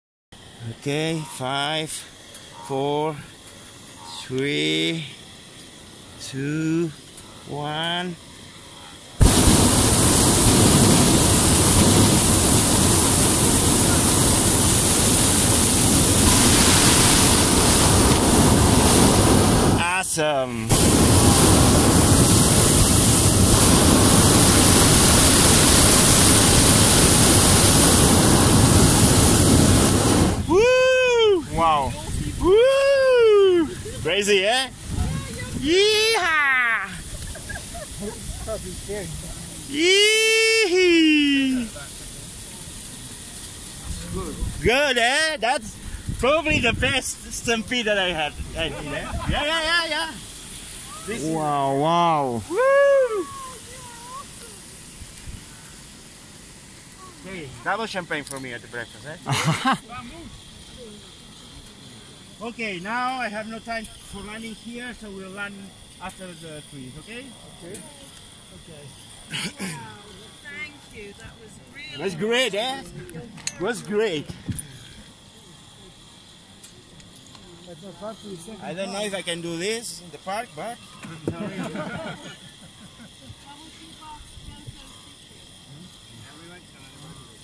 Impressive flight over wildebeest herd in Masai Mara, air balloon safari
Aug 15, 2018 - air balloon flight over Masai Mara. When we were getting close to the animals they heard the air balloon and went to stampede